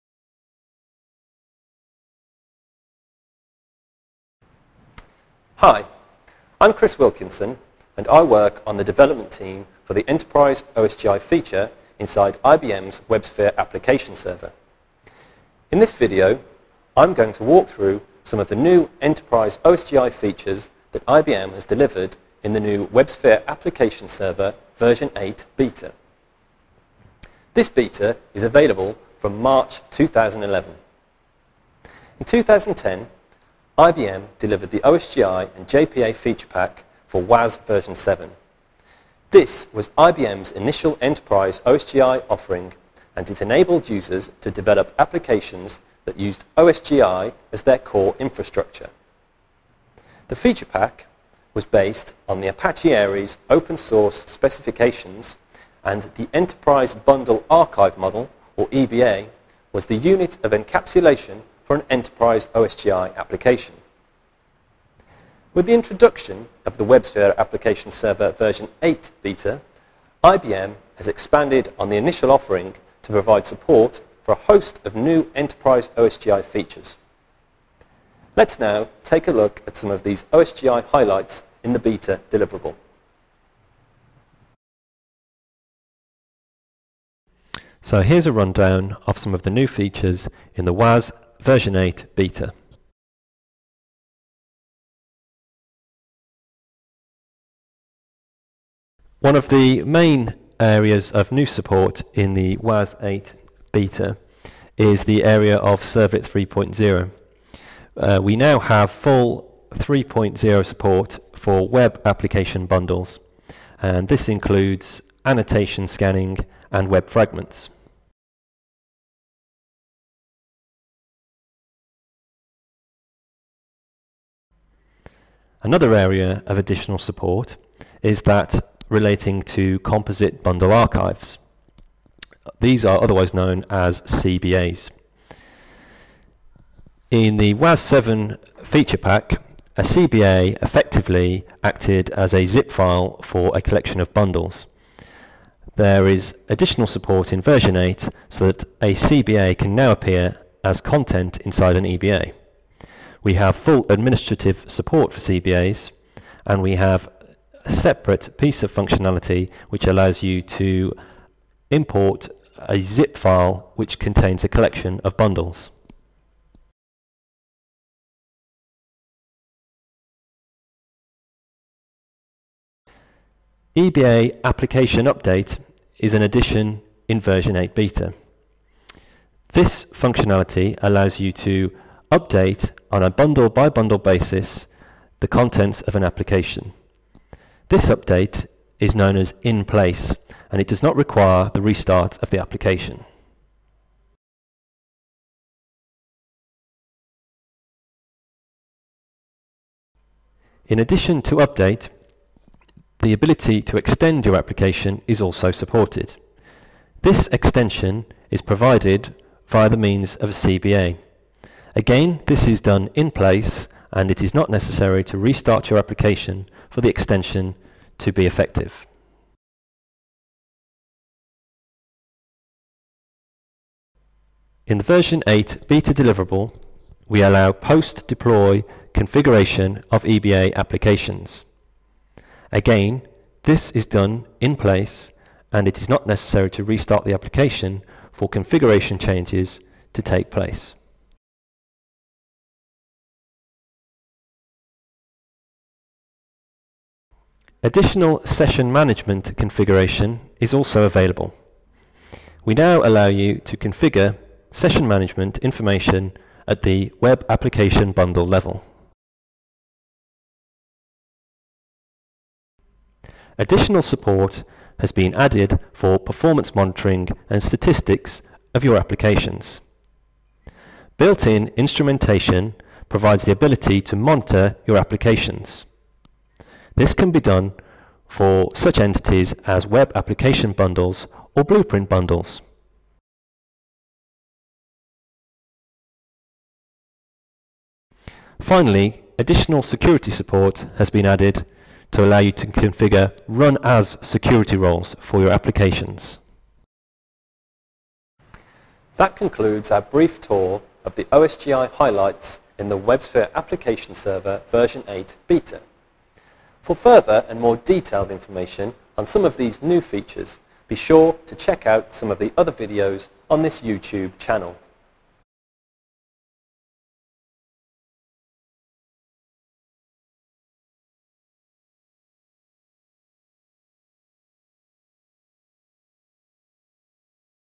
OSGi Applications New Features in WAS v8.0 Beta
Chris Wilkinson describes some of the key OSGi Applications features introduced in the new WAS v8.0 Beta